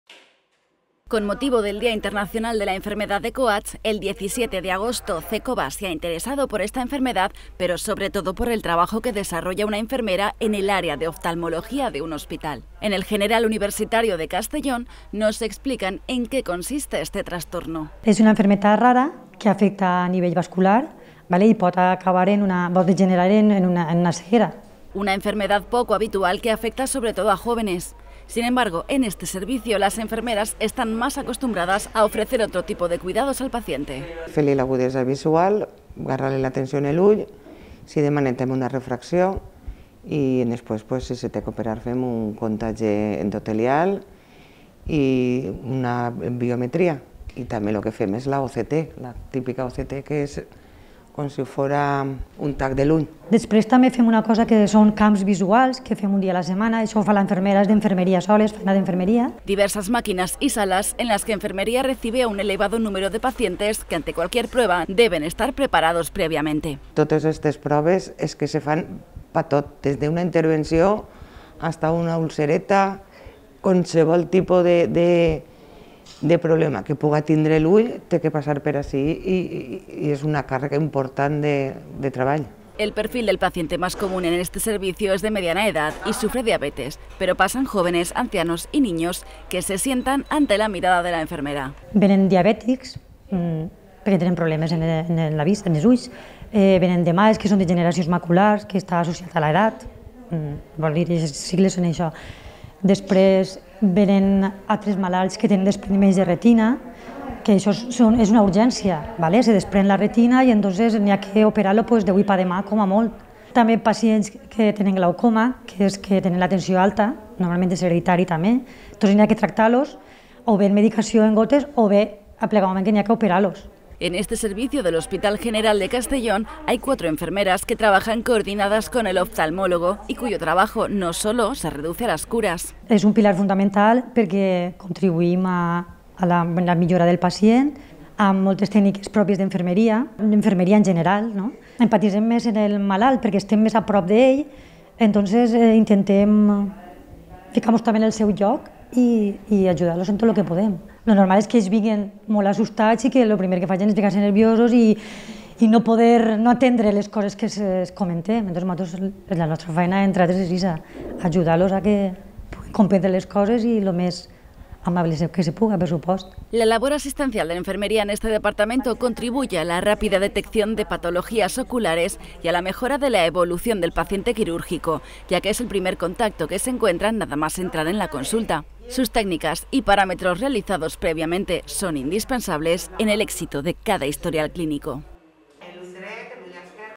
Enfermería como engranaje de la consulta de Oftalmología | Cecova TV
La enfermera en la consulta de oftalmología cumple una función muy importante y es responsable de la recuperación de muchos de los pacientes que acuden a ella. Desde la toma de muestras, realización de pruebas o cuidados y curas post quirúrgicas, un compendio de labores que hacen que el ritmo de visitas a la consulta sea fluido y el éxito de la mejora de la salud ocular de los pacientes una realidad.